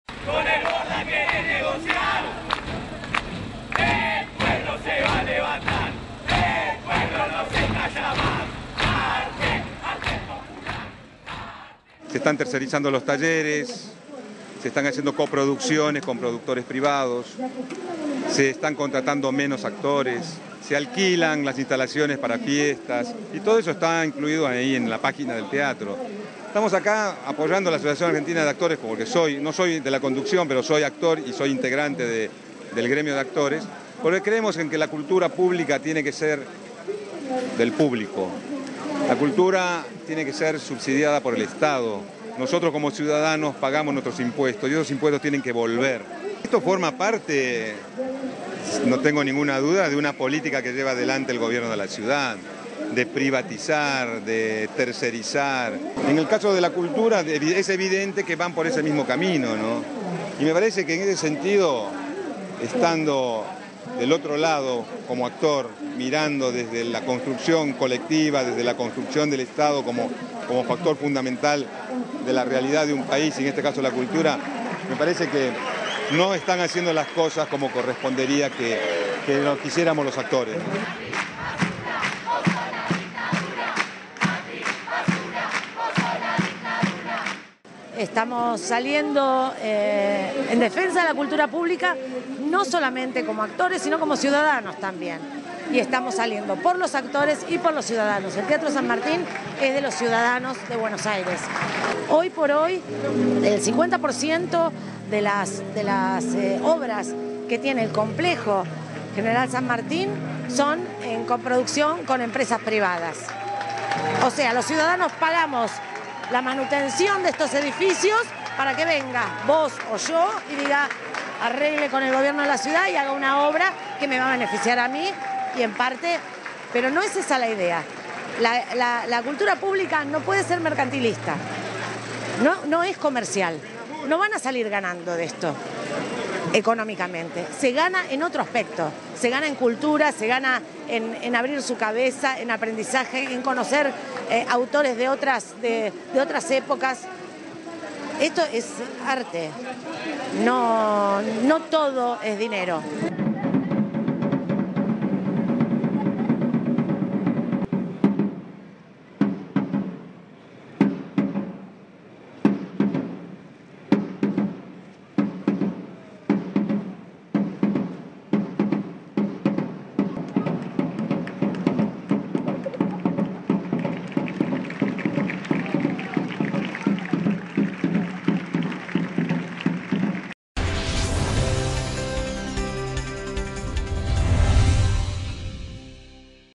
Actores porteños protestaron contra el vaciamiento del Teatro San Martín
Actores de la ciudad de Buenos Aires alertaron hoy sobre el deterioro de la política cultural porteña y advirtieron, en el marco de una jornada de movilización y protesta convocada por la Asociación Argentina de Actores, sobre la destrucción de bienes y recursos del Teatro General San Martín.
Centenares de artistas y público en general participaron esta tarde de la concentración frente al teatro, en avenida Corrientes al 1500, espacio que "contaba con equipos de trabajo y técnicos, maquinarias, utilería, sastrería, luminotecnia y otros rubros que van deteriorándose en forma alarmante sin que se haga nada para detener ese proceso", afirmó el actor Juan Palomino.

"El 50 por ciento de las obras son coproducidas por empresas privadas, ésa no es la finalidad del Teatro San Martín", conisderó Alejandra Darín, presidenta de la Asociación Argentina de Actores (AAA).
La protesta se realizó bajo la consigna "Con la cultura tampoco se negocia", sostenida por la Asociación Argentina de Actores.
"El San Martín ha sufrido la drástica reducción en la programación propia, la coproducción de obras con empresas, los contratos artísticos basura, la tercerización de los talleres (de sastrería, escenografía) y el alquiler del espacio para fiestas y eventos. Una serie de hechos que da como resultado una privatización encubierta", denunció un comunicado difundido por la Asociación.